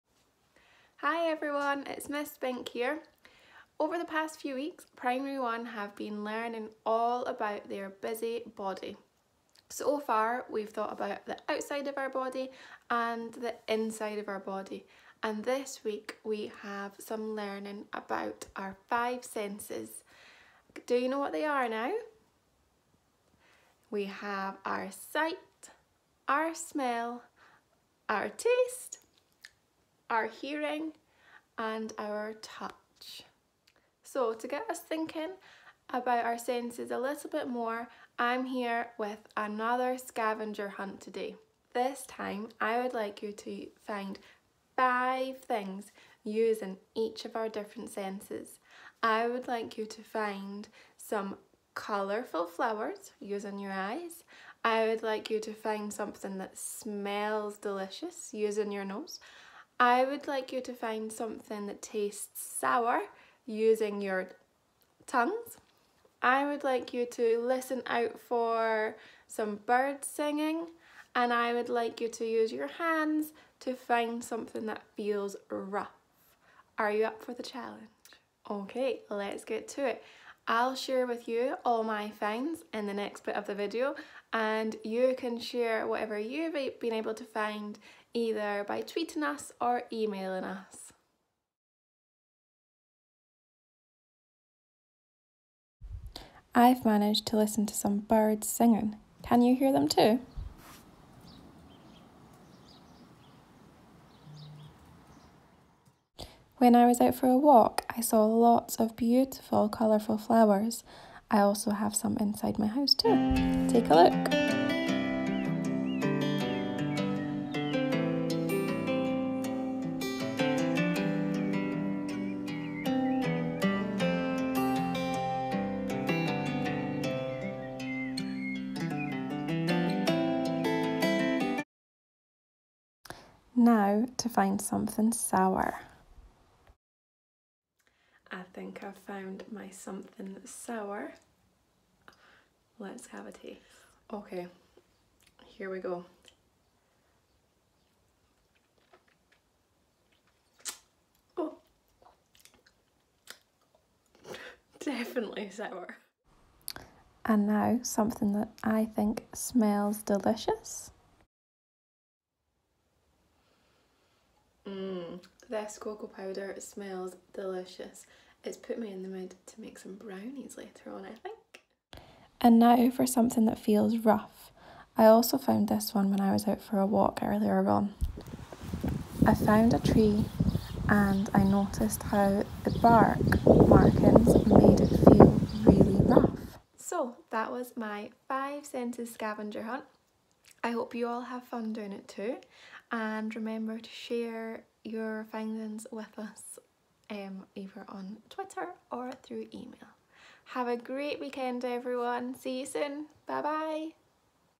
5 senses scavenger hunt
P1 have been learning about the 5 senses. This video helps us to think about them a bit more 😊